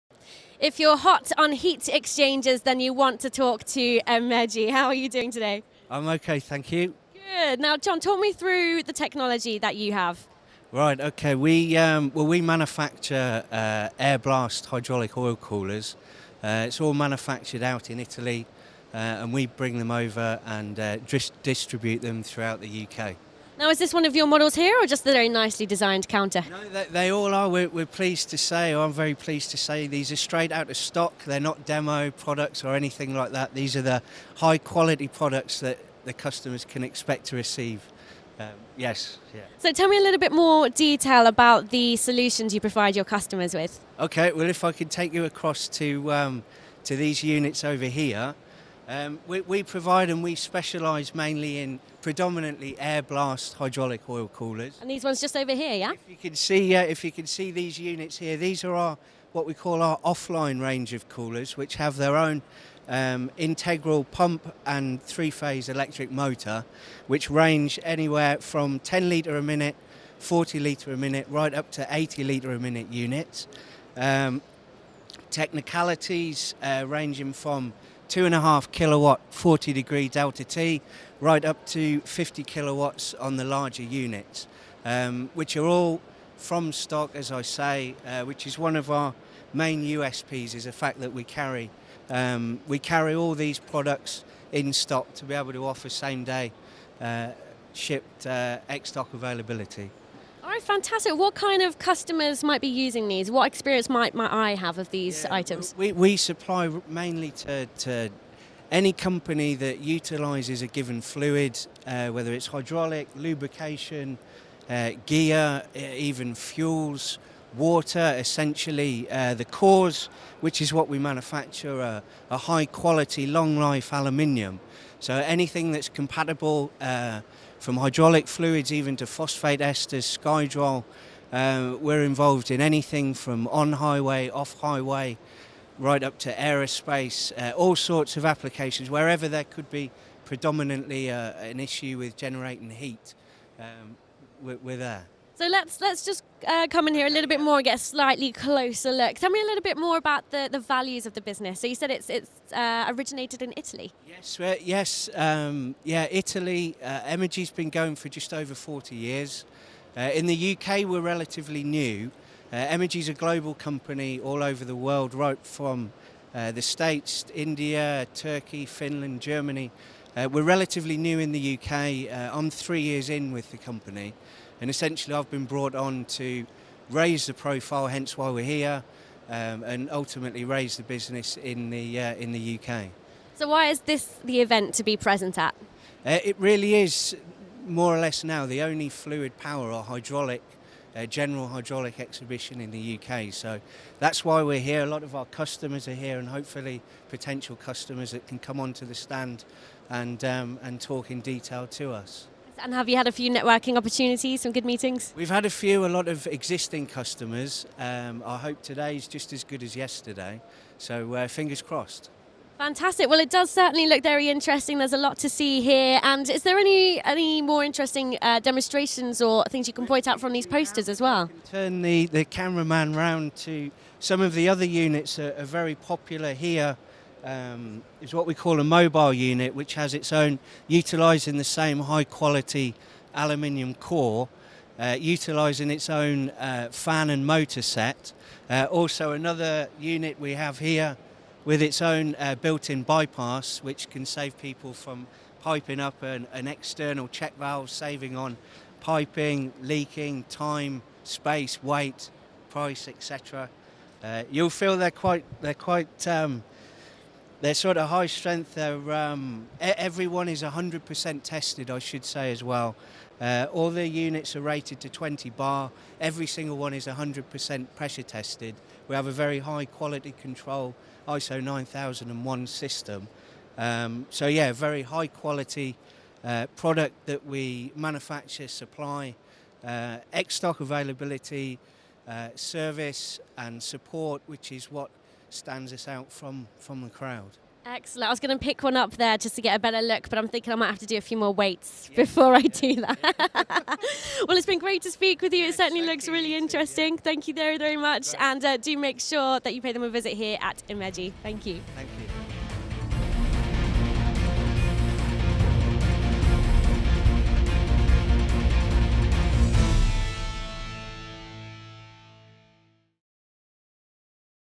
17 T FPS16 Emmegi Heat Exchangers v1 Youtube
Emmegi’s ) range of products spans air cooled, water cooled, and oil cooled heat exchangers for industrial and mobile applications. Additionally, beyond our line of heat exchanger standards, we design and manufacture custom combination air/water/oil coolers for intricate application requirements.